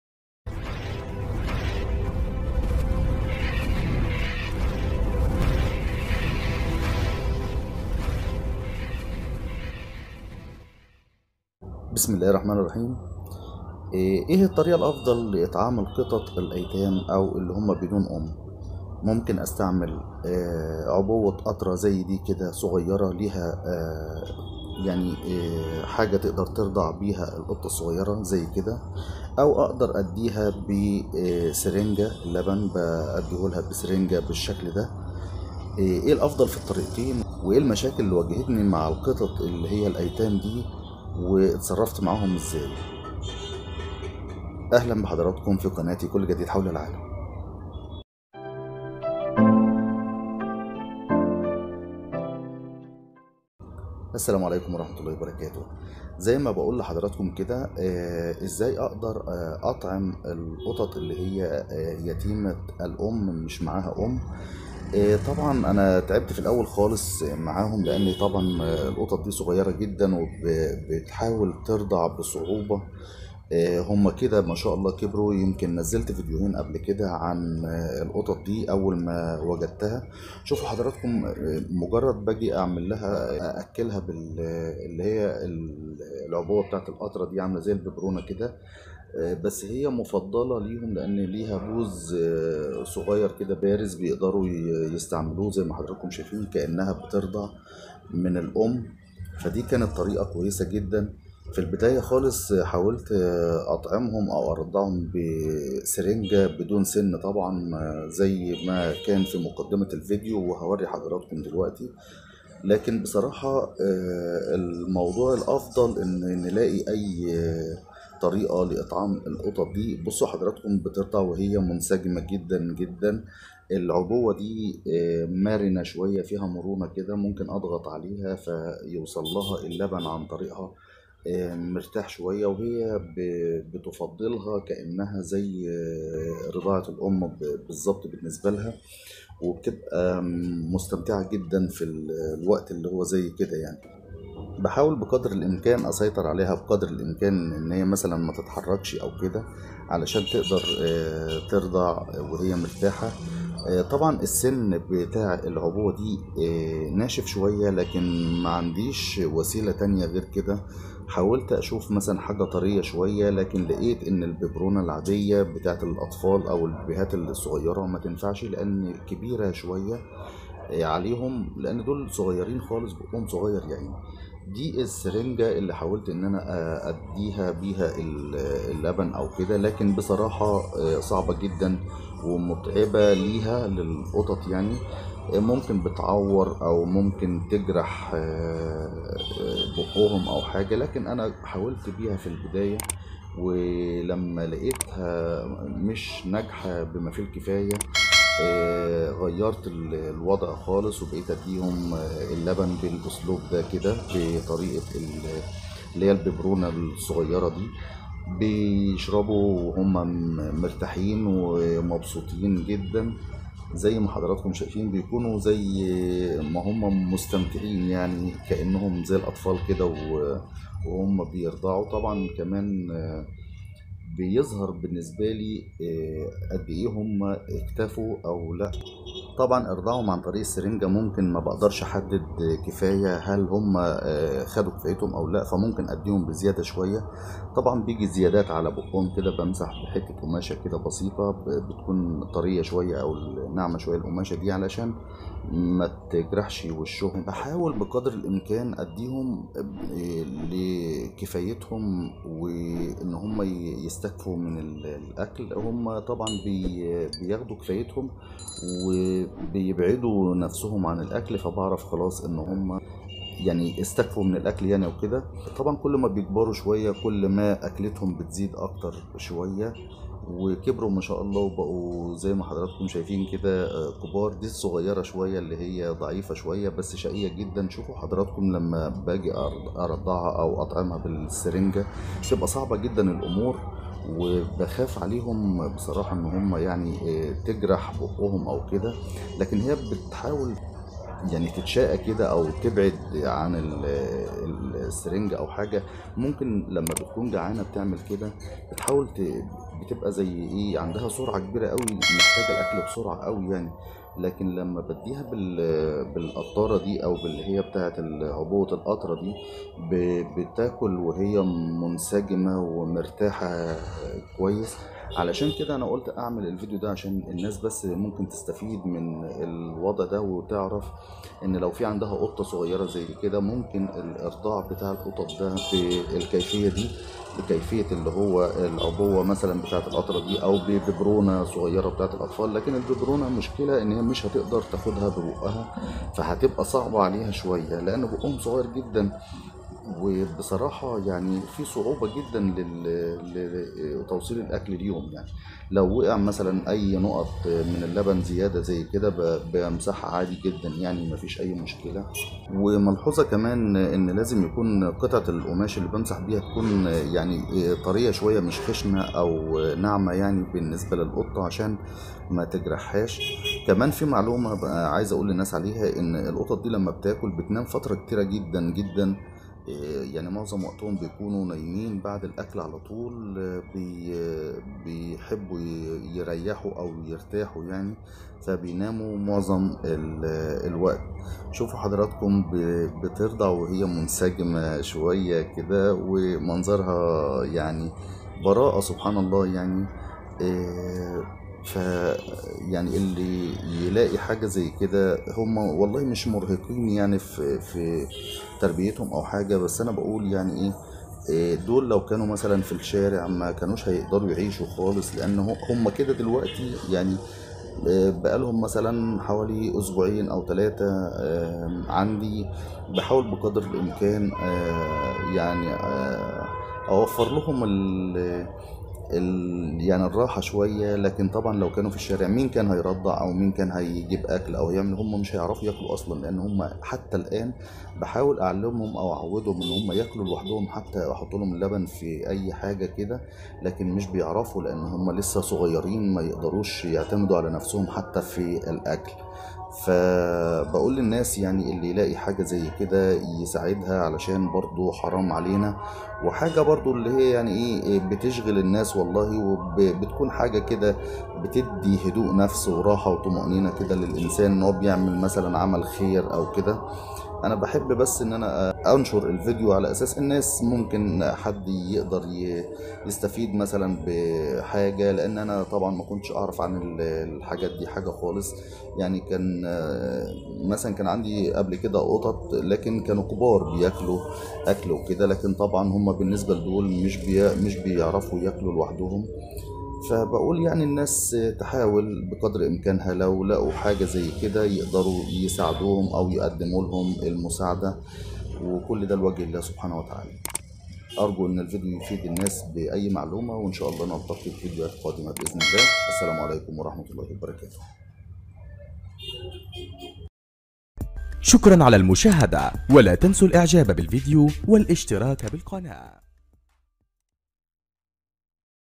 كيفية العناية بالقطط الصغار عمرها أيام 😸_ هل السرنجة أفضل أم ببرونة صغيرة ؟ شاهدوا التجربة الفعلية
فضلا و ليس أمرا إشترك بالقناة و فعل زر الجرس ليصلك كل جديد دائما من قناتى